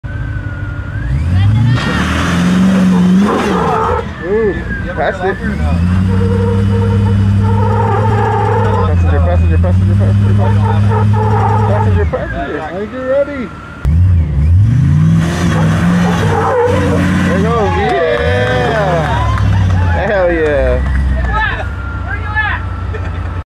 Land Rover vs Right Monkey
Not every day you see a Land Rover out wheeling .